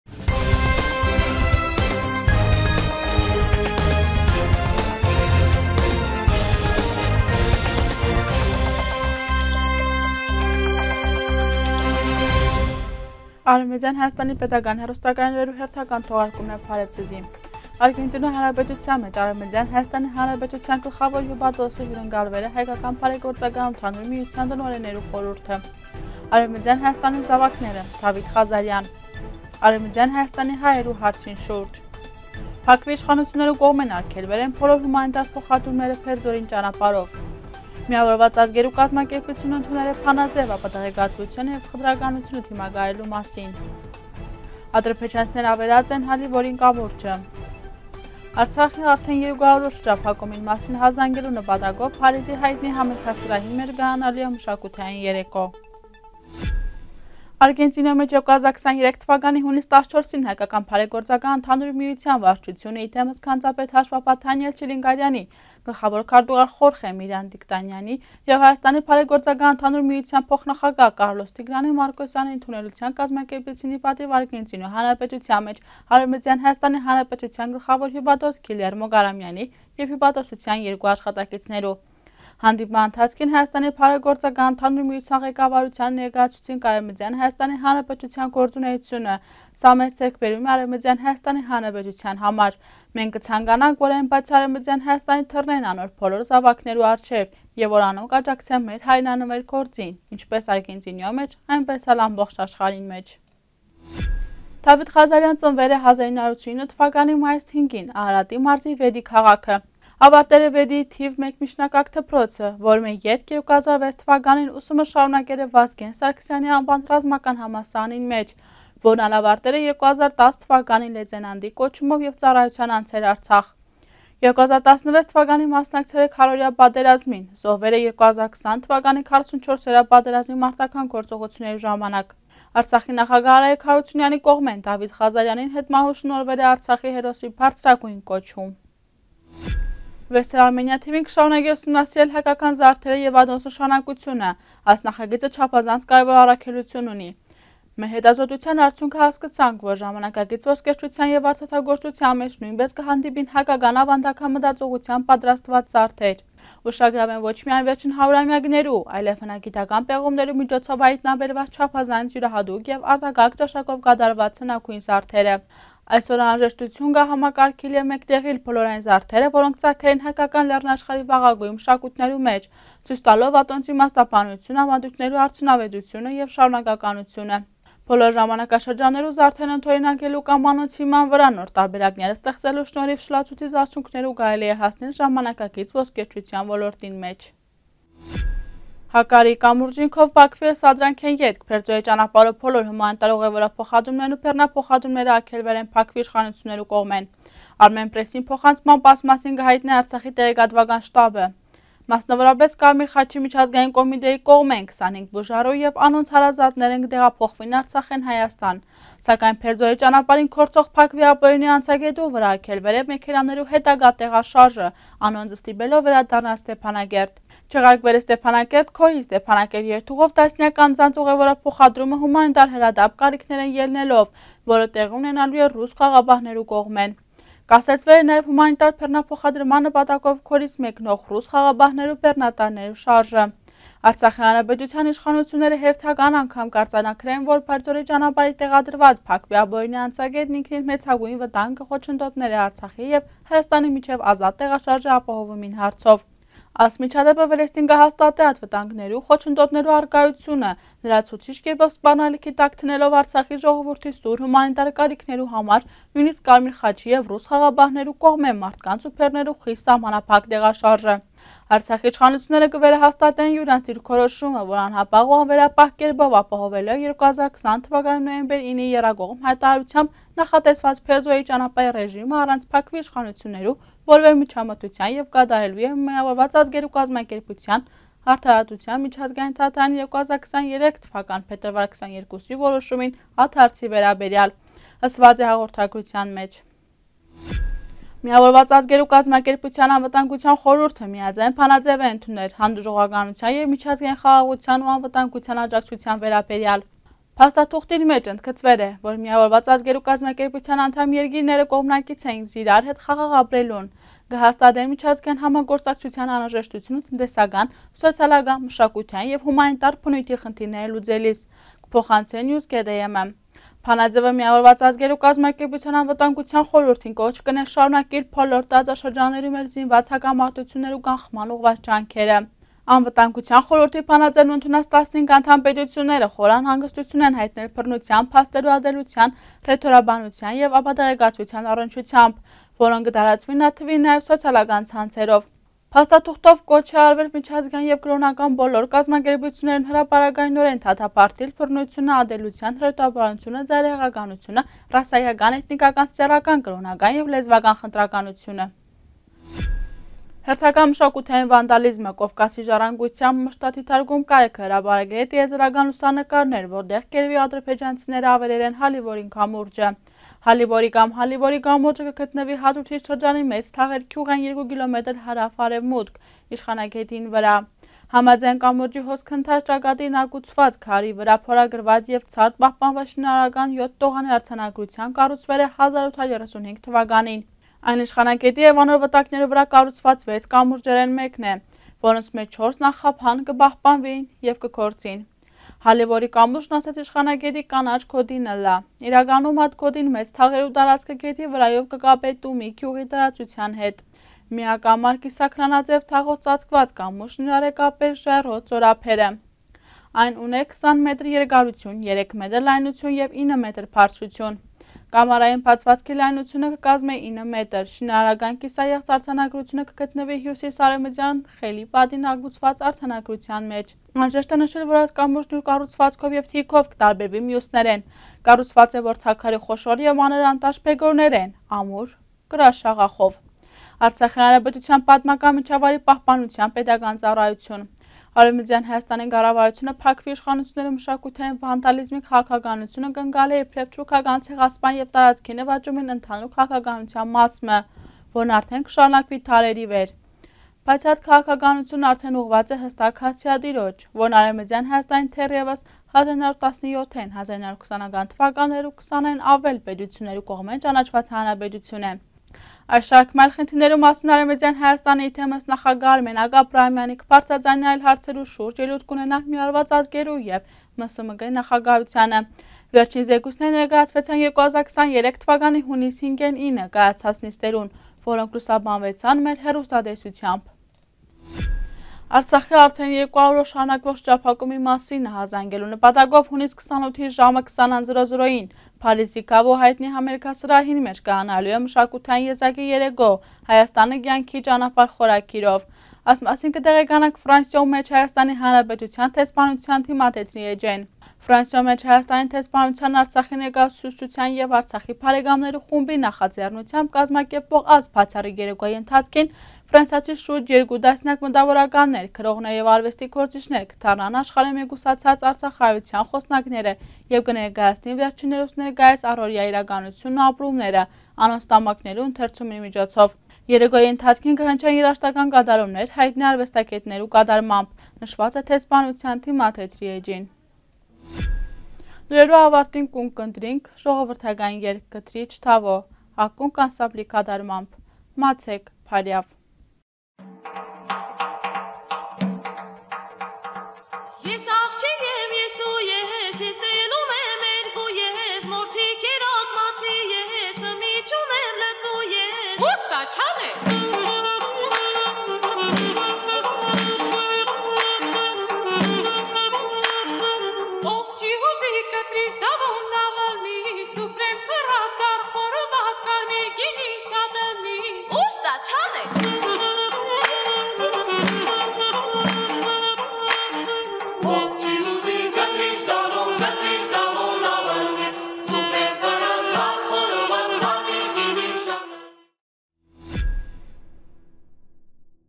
Արեւմտեան Հայաստանի զաւակները․ Դավիթ Ղազարեան
1. Արգենտինիոյ Հանրապետութեան մէջ  Արեւմտեան Հայաստանի Հանրապետութեան գլխաւոր հիւպատօսը հիւրընկալուեր է Հայկական բարեգործական ընդհանուր  միութեան տնօրէններու խորհրդը
2. Արեւմտեան Հայաստանի զաւակները․ Դավիթ Ղազարեան
3. Արեւմտեան Հայաստանի հայերու հարցին շուրջ
4. Բաքուի իշխանութիւններու կողմէն արգելուեր են բոլոր հումանիտար փոխադրումները Բերձորի ճանապարհով
5. ՄԱԿ-ն ընդուներ է բանաձեւ ապատեղեկատւութեանը եւ խտրականութեանը դիմակայելու մասին
6. Ադրբեջանցիներն ավերած են Յալիւորի կամուրջը
7. Արցախի՝ արդեն 200 օր շրջափակումին մասին ահազանգելու նպատակով Փարիզի յայտնի համերգասրահին մէջ  կայանալու է մշակութային երեկոյ